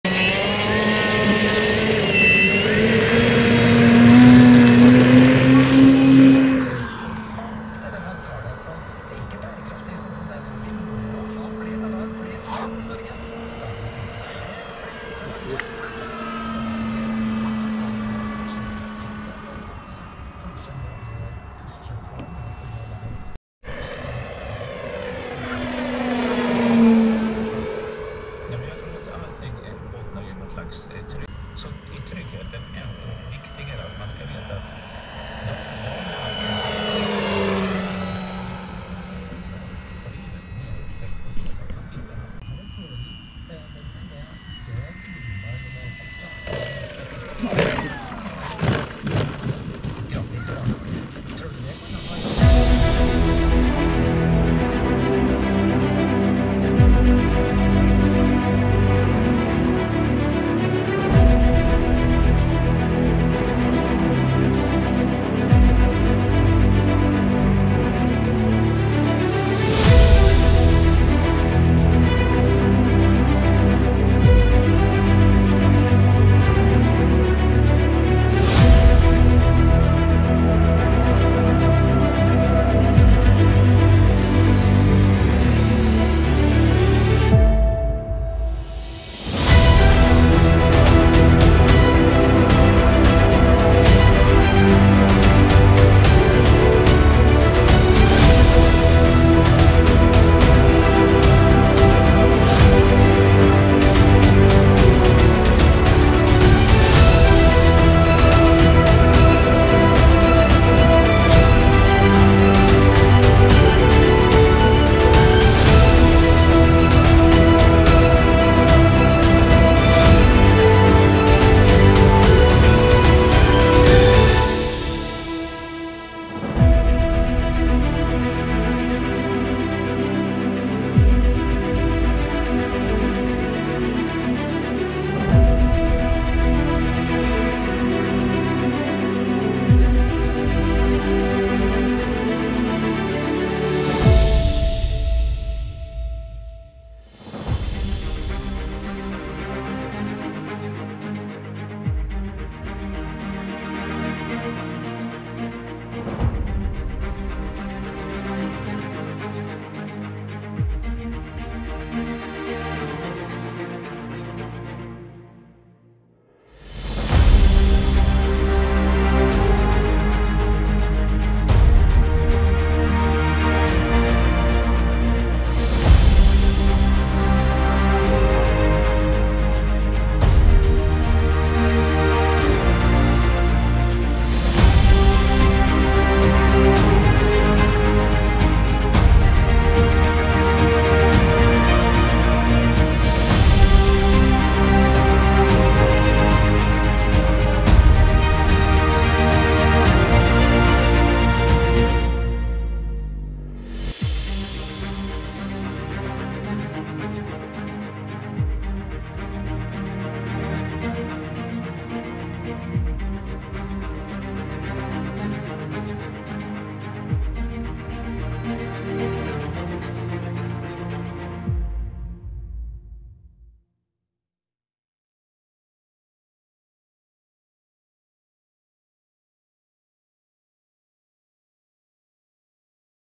IMFK - Chasing E-Flite T-28 Trojan w/DJI FPV + GoPro Hero 8
Finally we got the chance to test out the GoPro Hero 8 mounted on the DJI FPV. No significant drop in performance on the drone, flighttime only slightly reduced, way better video quality and the option to edit slowmo without dropping frames. 3D printed mount, see no reason to upgrade drone or gopro for the time being. A bit annoying with droplets on the cameras, but at least we got the testing done :)